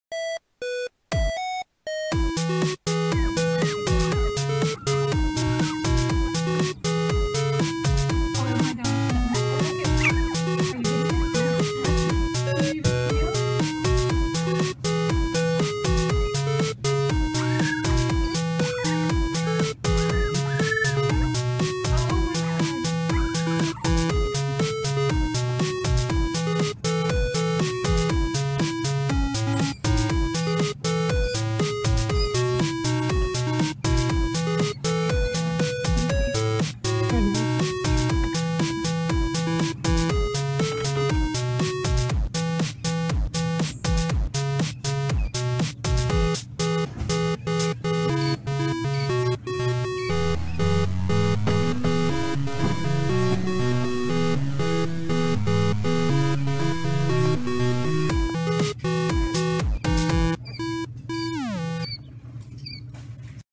Puppy & Quail
Introducing 2 week old puppies to 2 week old quail.